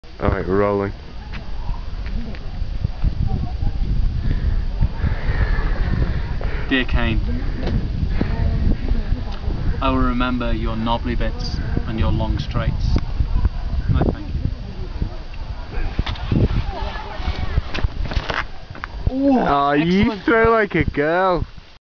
Marky's Stick
Mark bidding adieu to his walking stick after two days of loyal service.